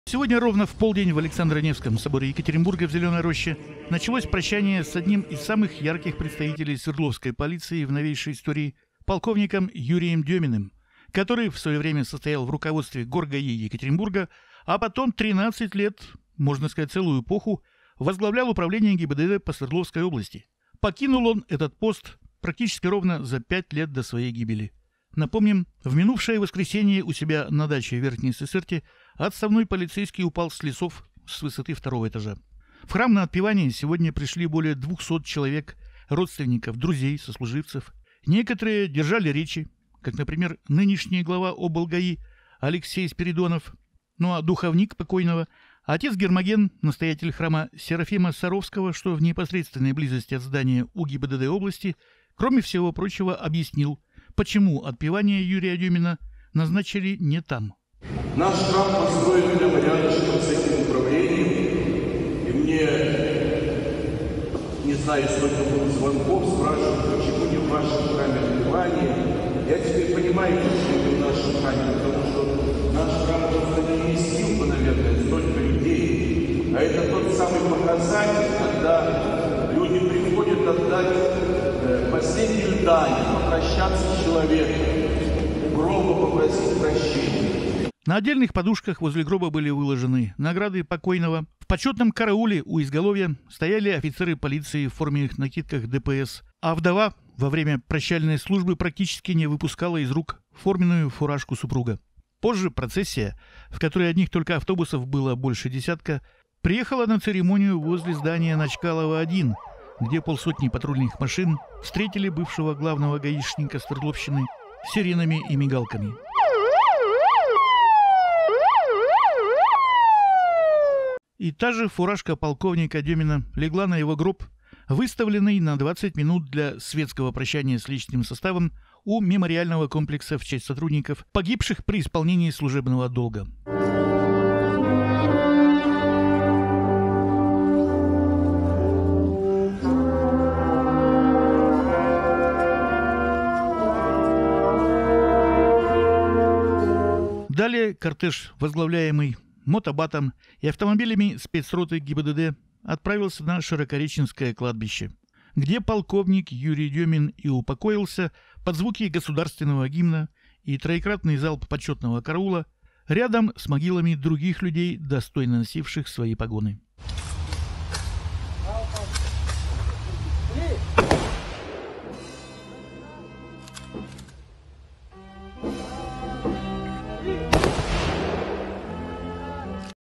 Похороны экс-начальника ОблГАИ.
Юрия Дёмина отпели и отдали воинские почести.